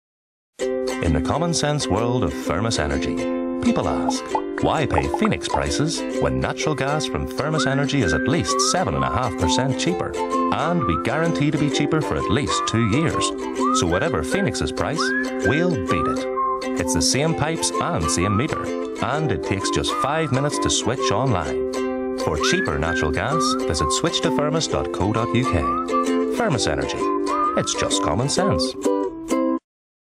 Switch to firmus TV Ad for Greater Belfast
firmus energy's latest ad targeting homes in Greater Belfast currently using Phoenix as their gas supplier. firmus is offering a two year tracker deal giving savings of 7.5% in year one and 5% in year 2.